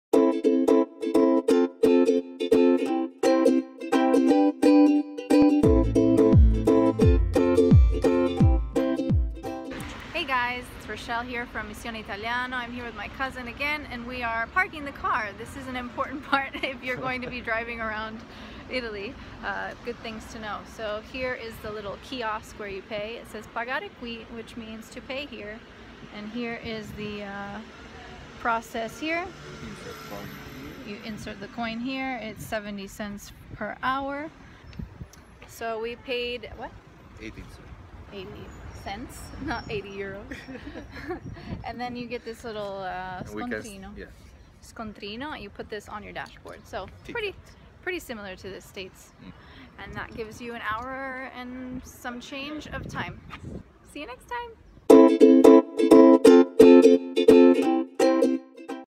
How to pay for parking in Italy
Planning to drive around Italy?  Today, we demonstrate how to pay for parking in the city center of Matera, Italy.